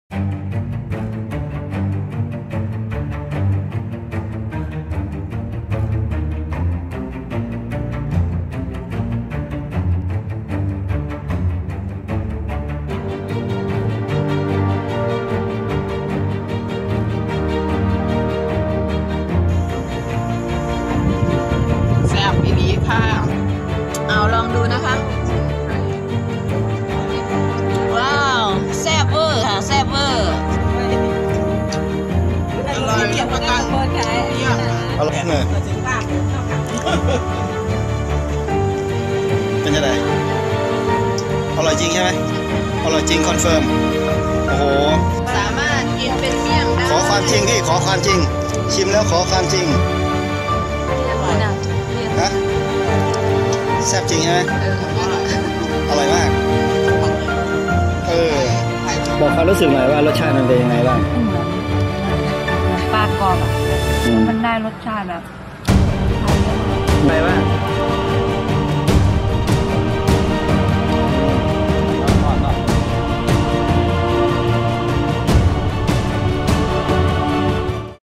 เมนูพิเศษ สำหรับคนรักสุขภาพ!!! พร้อมเสิร์ฟเป็นคำๆ ทานกับผักอะไรก็อร่อย
เหมี่ยงเห็ดสมุนไพร โรยหน้าด้วยหอมเจียวและกระเทียมเจียว ลองชิมแล้วจะติดใจ รสชาติจัดจ้านได้ใจ
ผลิตโดย : กลุ่มวิสาหกิจชุมชนเกษตรกรรมยั่งยืนอำเภอขุขันธ์ จังหวัดศรีสะเกษ